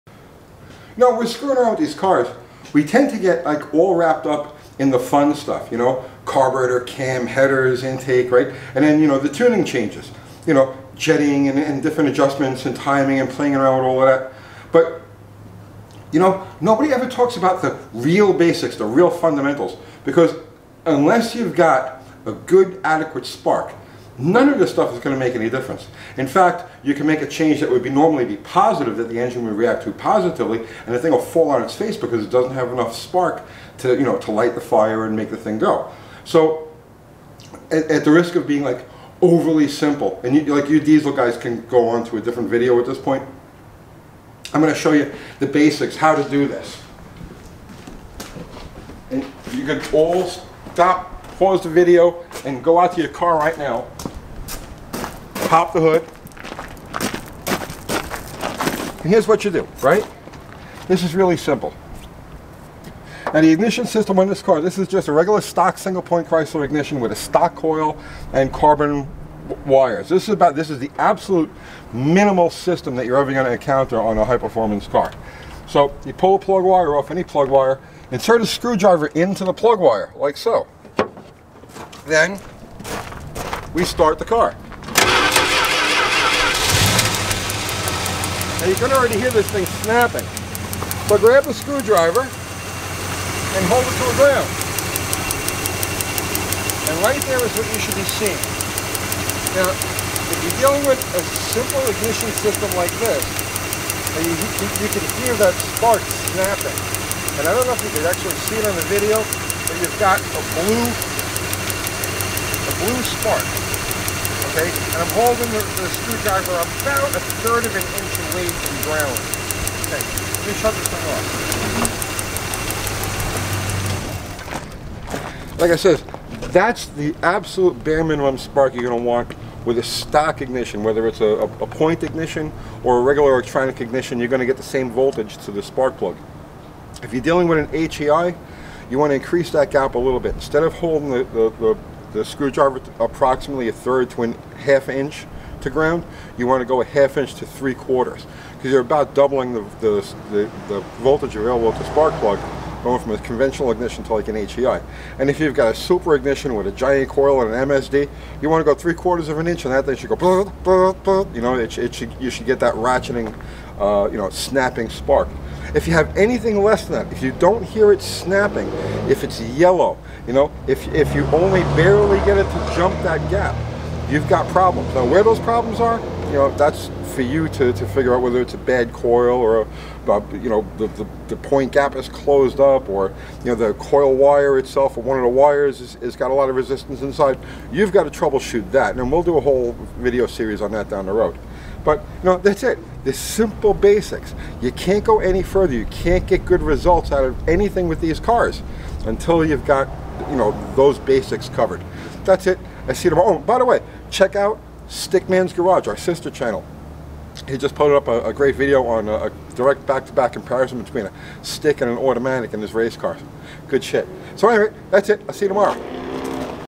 Engine Fundamentals- Checking Spark Quality
The only special equipment you'll need to know if your ignition system is up to snuff are your eyes ears and a screwdriver.